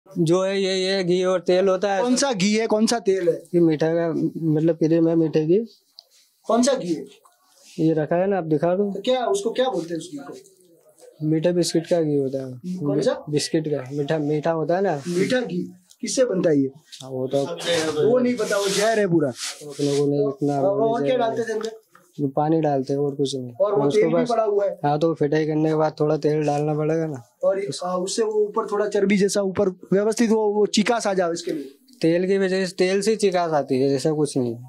Operation Asur: કેમિકલ નાખીને તૈયાર કરવામાં આવતું હતું માખણ | Gujarat FIRST
લોકોના સ્વાસ્થ્ય સાથે રમતા લોકોનો ગુજરાત ફર્સ્ટની ટીમે કર્યો પર્દાફાશ
કેમિકલ નાખીને તૈયાર કરવામાં આવતું હતું માખણ
વિવિધ વિસ્તારમાં પણ નકલી માખણનું ધૂમ વેચાણ